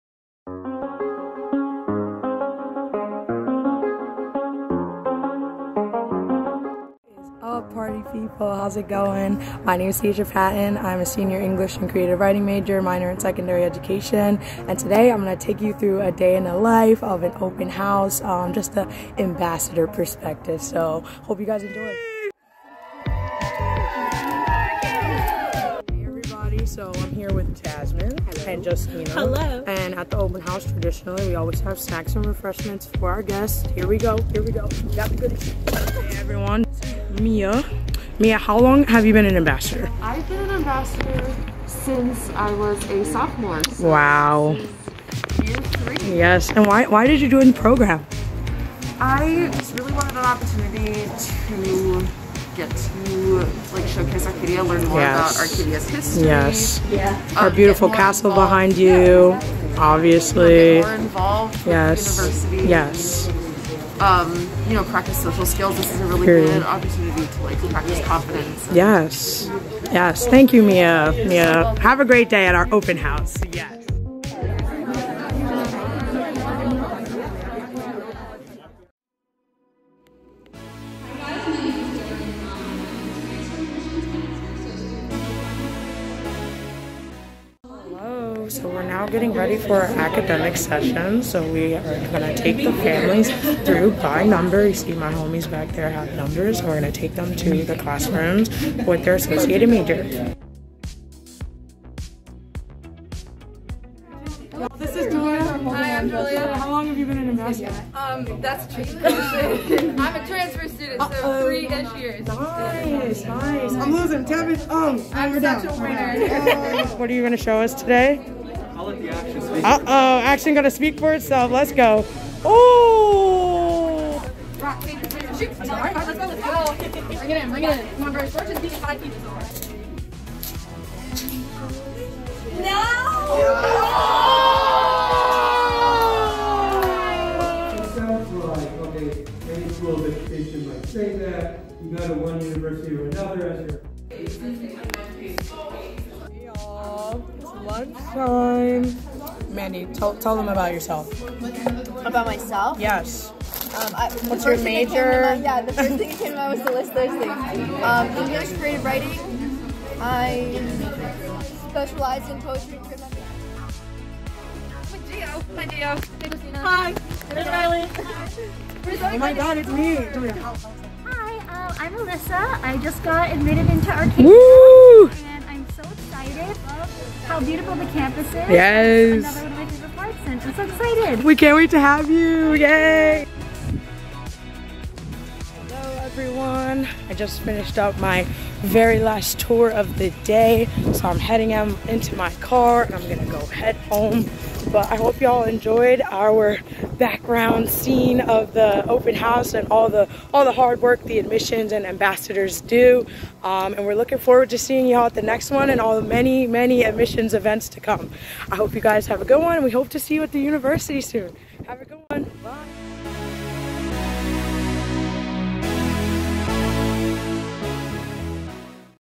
Open House at Arcadia University (Student Vlog)
By Daijah Patton '22: "Imagine: It’s been two years since you’ve worked at an Open House, and Arcadia finally starts having them again. As someone who’s been an Arcadia Ambassador for four years, there’s nothing better than giving families tours around our beautiful campus, directing people to sessions, and meeting new people interested in our University.

Watch as I take you through how the Ambassadors get amped up for the day, and how we keep it smooth sailing for all of our prospective students. Hope you enjoy it, and that you come visit us soon!"